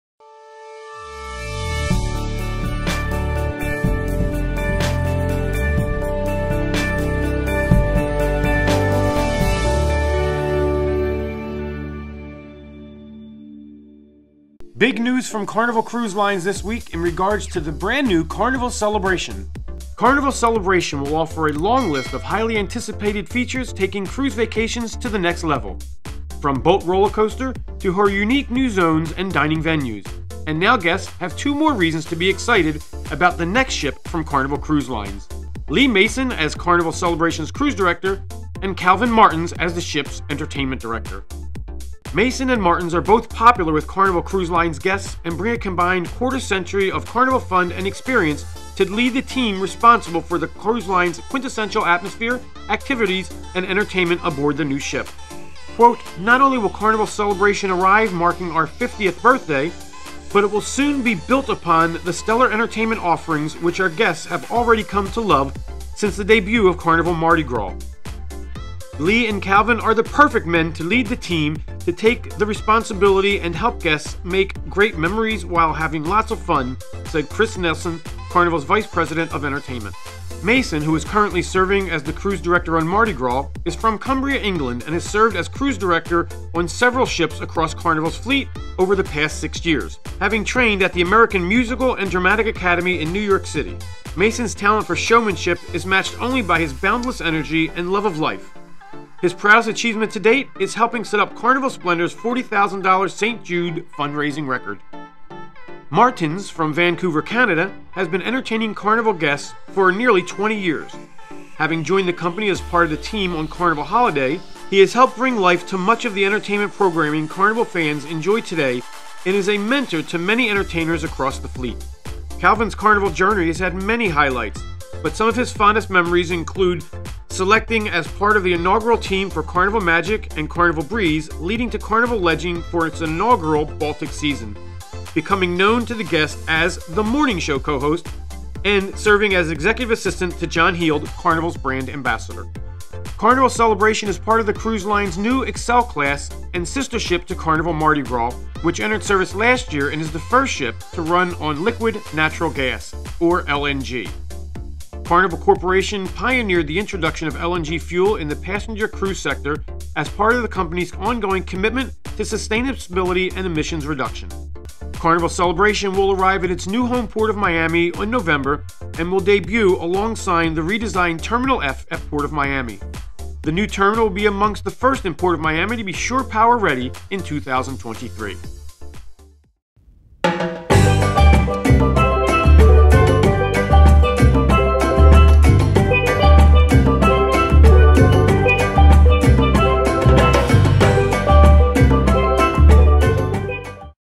Cruise and Entertainment Director Picked for the new Carnival Celebration.
Lee Mason Named New Ship’s Cruise Director, Calvyn Martens to Serve as Entertainment Director

Carnival Celebration will offer a long list of highly anticipated features taking cruise vacations to the next level, from her BOLT rollercoaster to her unique new zones and dining venues, and now guests have two more reasons to be excited about the next new ship from Carnival Cruise Line: Lee Mason as Carnival Celebration’s cruise director and Calvyn Martens as the ship’s entertainment director.

Mason and Martens are both popular with Carnival guests and bring a combined quarter-century of Carnival fun and experience to lead the team responsible for the cruise line’s quintessential atmosphere, activities and entertainment aboard its new ship.

“Not only will Carnival Celebration arrive as the ship marking our 50th birthday, but it will build upon the stellar entertainment offerings which our guests have already come to love since the debut of Mardi Gras. Lee and Calvyn are the perfect team to take on this responsibility and help our guests make great memories while having lots of fun,” said Chris Nelson, Carnival’s vice president of entertainment.

Mason, currently serving as cruise director on Mardi Gras, is from Cumbria, England, and has served as cruise director on several ships across Carnival’s fleet over the past six years, including the return to service of Carnival Panorama in Long Beach. Having trained at the American Musical & Dramatic Academy in New York City, Mason’s talent for showmanship is matched only by his boundless energy and love for life. His proudest achievement to date is helping to set Carnival Splendor’s $40,000 St. Jude fundraising record.

Martens, from Vancouver, Canada, has been entertaining Carnival guests for nearly 20 years, having joined the company as part of the team on Carnival Holiday. He has helped bring to life much of the entertainment programming Carnival fans enjoy today and is a mentor to many entertainers across the fleet. Calvyn’s Carnival journey has had many highlights, but some of his fondest memories include being selected as part of the inaugural team for Carnival Magic and Carnival Breeze, leading the Carnival Legend for its inaugural Baltic season, becoming known to guests as the “Morning Show” co-host and serving as Executive Assistant to John Heald, Carnival’s brand ambassador.

Carnival Celebration is part of the cruise line’s new Excel-class and the sister ship to Mardi Gras, which entered service last year as the first ship in North America to run on an Liquefied Natural Gas (LNG) propulsion system. Carnival Corp. pioneered the introduction of LNG fuel in the passenger cruise sector as part of the company’s ongoing commitment to sustainability and emissions reduction.

Carnival Celebration will arrive to its homeport, Miami, in November and will debut along with the redesigned Terminal F at PortMiami. The new terminal will be among the first at PortMiami to be shore-power ready in 2023.